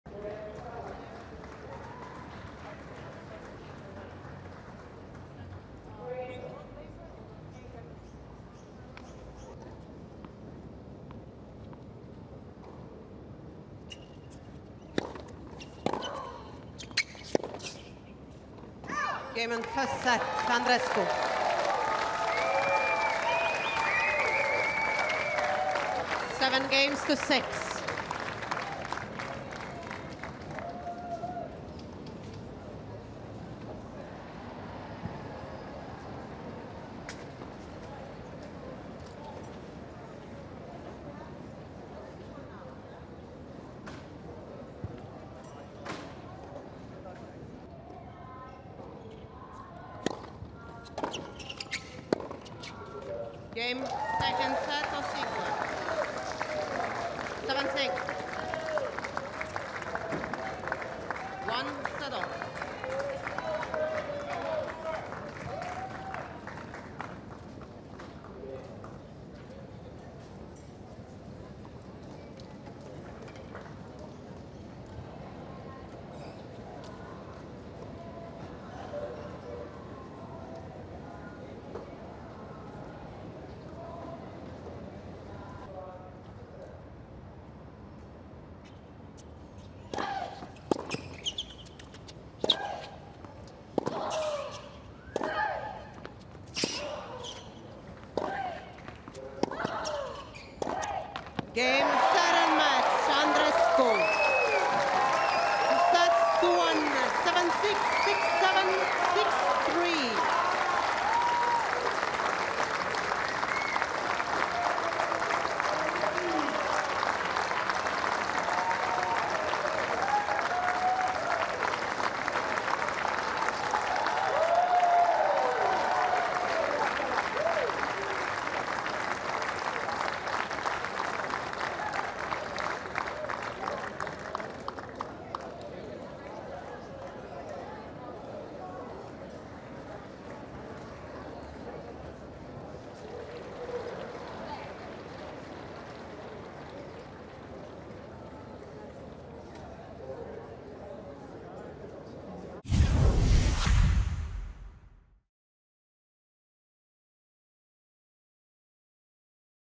Bianca Andreescu v Whitney Osuigwe match highlights (1R) | Australian Open 2019
Bianca Andreescu v Whitney Osuigwe match highlights (1R)